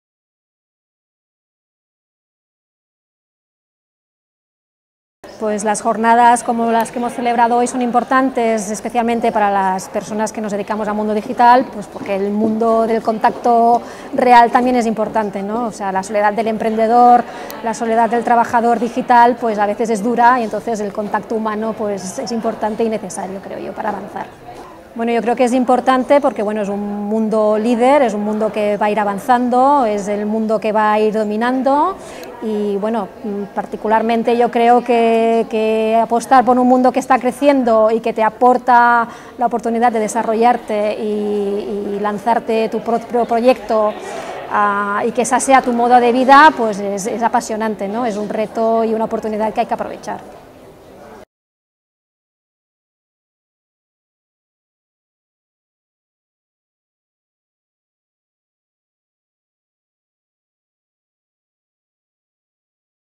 "Apostar por un mundo que crece es apasionante"I Xarxa Punt TIC
Yolanda Bonilla és fundadora i CEO de miximoms.com. Ha participat a la taula "Dones emprenedores liderant la Transformació Digital" dins de les Jornades Internacionals de Dones Liderant les TIC WomanLiderTIC.